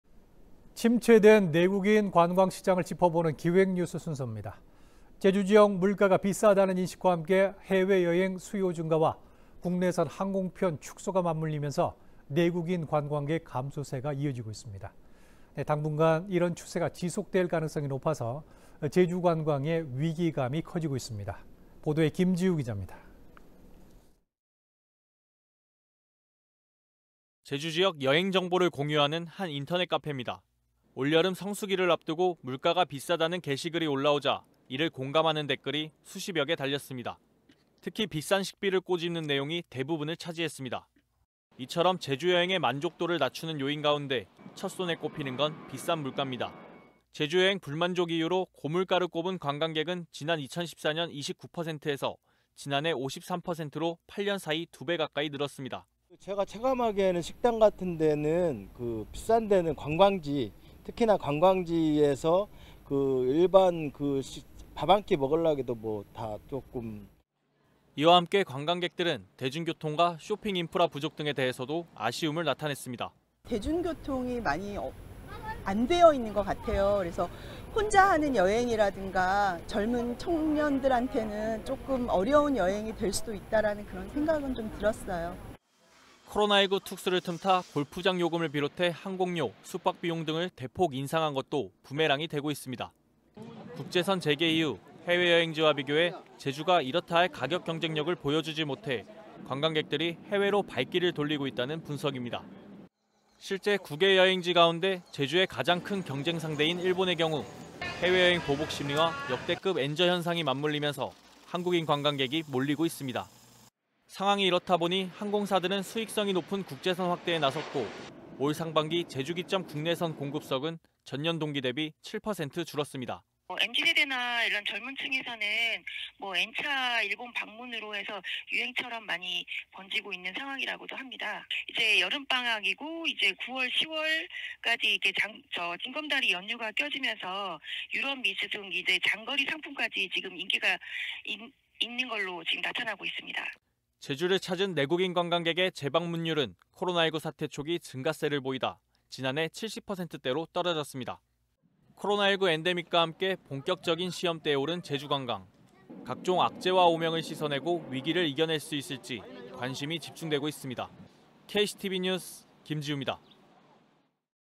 [내국인 관광시장 침체 ②] 엔데믹 시대, 제주관광객 왜 줄었나?
침체된 내국인 관광시장을 짚어보는 기획뉴스 순서입니다.

제주지역 물가가 비싼다는 인식이 여전한 가운데 해외여행 수요 증가와 제주 기점 국내선 항공편 축소 등이 맞물리면서 내국인 관광객 감소세가 이어지고 있는 것으로 나타났습니다.

이같은 현상이 지속될 가능성을 배제할 수 없어 관광산업의 위기감이 커지고 있습니다.